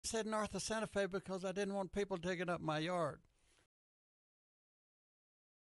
Forrest Fenn, I Said North Of Santa Fe Because I Didn't Want People Digging Up My Yard
Forrest Fenn Treasure, I Said North Of Santa Fe Because I Didn't Want People Digging Up My Yard